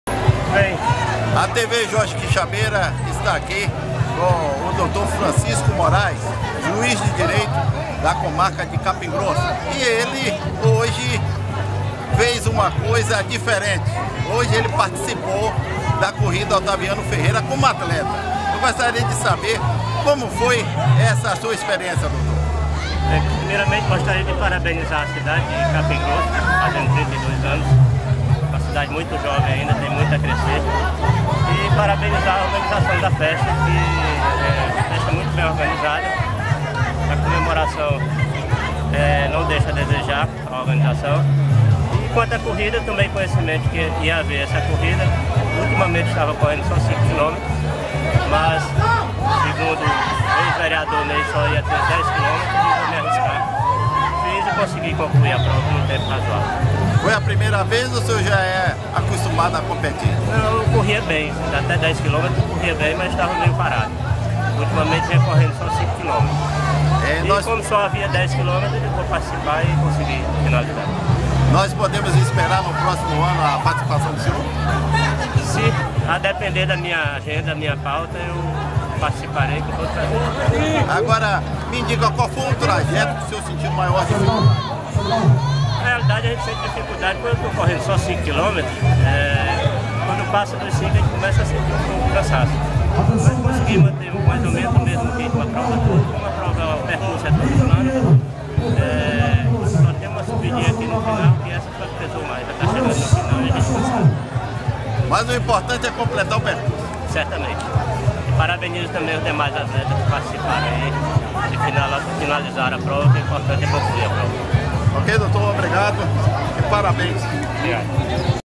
TV JORGEQUIXABEIRA ENTREVISTA O MERENTÍSSIMO JUIZ DR. FRANCISCO QUANDO DA CORRIDA OTAVIANO FERREIRA
TV JORGEQUIXABEIRA ENTREVISTA O MERITÍSSIMO JUIZ DR. FRANCISCO QUANDO DA CORRIDA OTAVIANO FERREIRA